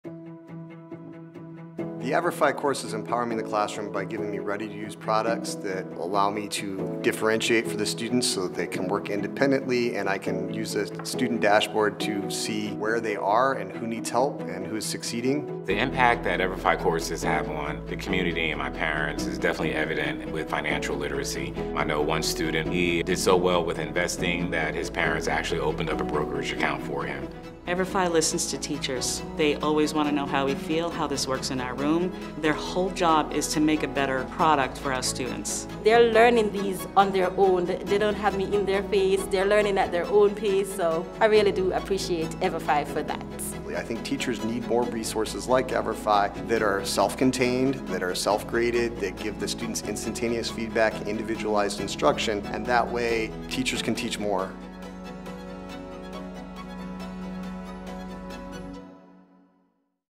EVERFI Teacher Community - Our Why
Hear from teachers on the impact EVERFI courses are having in their classroom and in their communities.